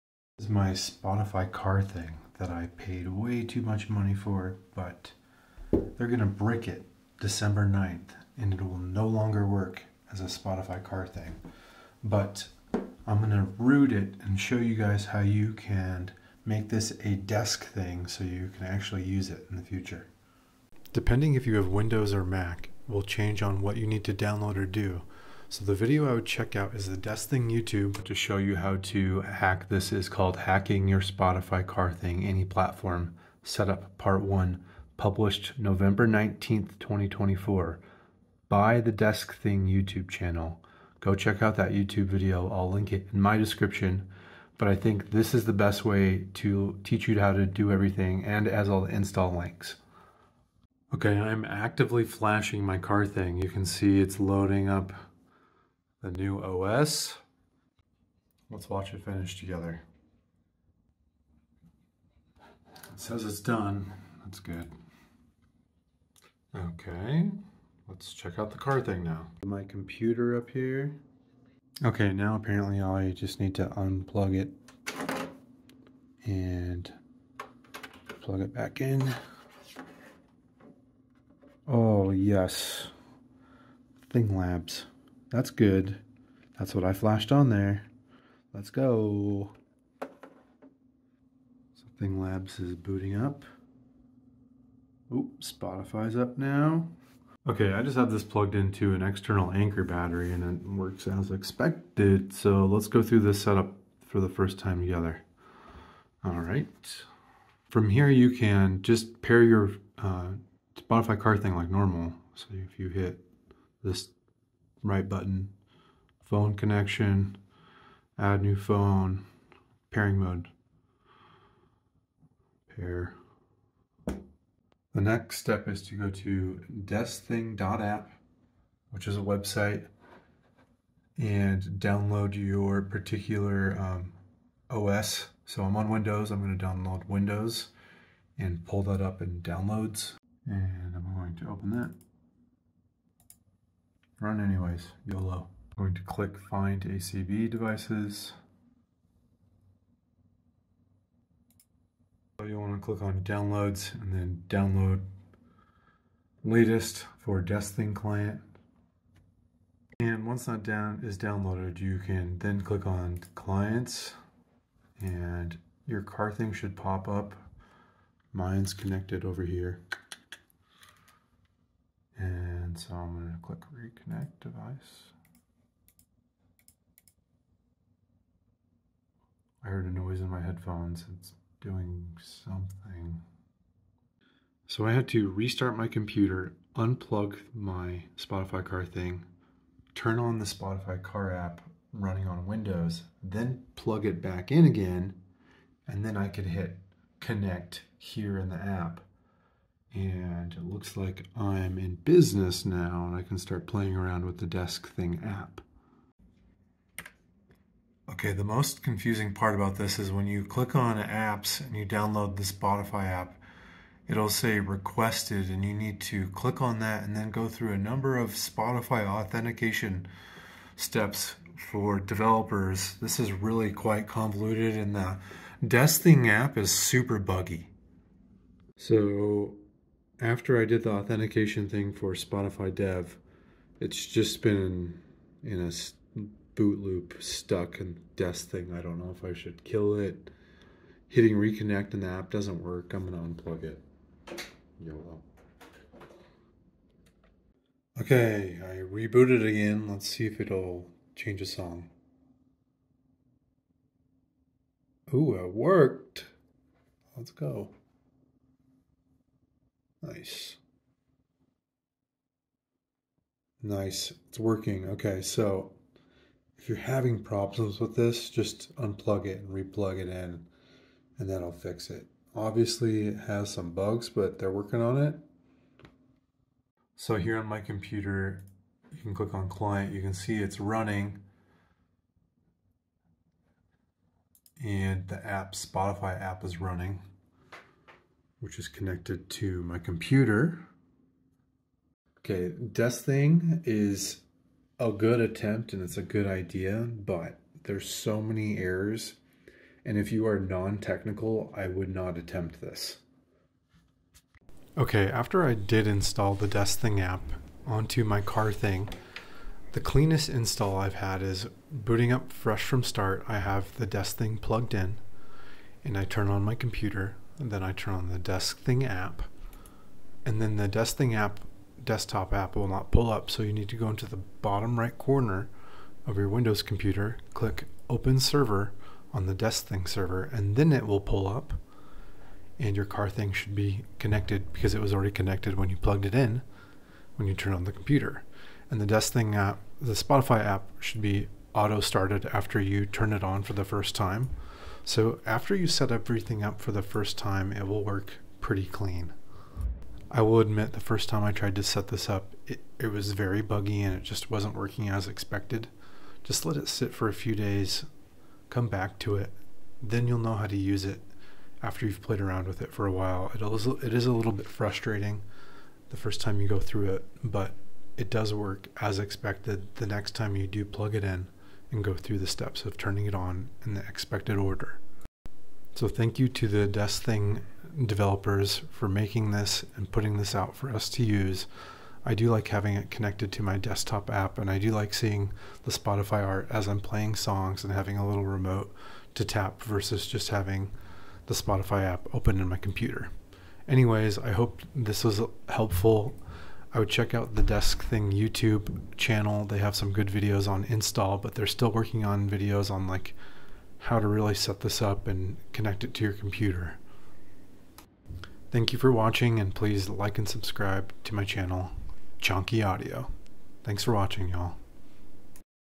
How I updated my Spotify Car thing to "Desk Thing" Software on Windows OS
After December 9th, 2024, Car Thing will be discontinued and will no longer be operational. As of now, there are no plans to release a replacement or new version of Car Thing. Contact customer service by no later than January 14, 2025 to discuss your refund options. I got mine on Ebay, so I was not able to get a refund - So I was incentivized to figure out how to turn my carthing into a deskthing!

I created this video to show how I went about installing DeskThing Software on my device. I do not mean to take credit for the how to video below on the rooting steps specifically. But I wanted to share my overall experience with people and the bugs / tricks I learned along the way on installation specifically on Windows operating system.

Link to Desk Thing video which shows you how to Root your Spotify Car Thing device (Windows or MacOS) - Found on DeskThing youtube Channel

After you install the Thing Labs OS, I think my video is a nice guide on how to quick start from there.

DeskThing software has been really buggy for me. I would not attempt this if you do not consider yourself technical. But overall isnt that bad in my opinion.

Tool download links used in video above.
deskthing.app - Download desk thing desktop app

Timestamps if interested:
How to root Car Thing in Burn Mode - 0:20
deskthing.app desktop app - 2:04
How to boot DeskThing Software on Spotify Car Thing - 5:04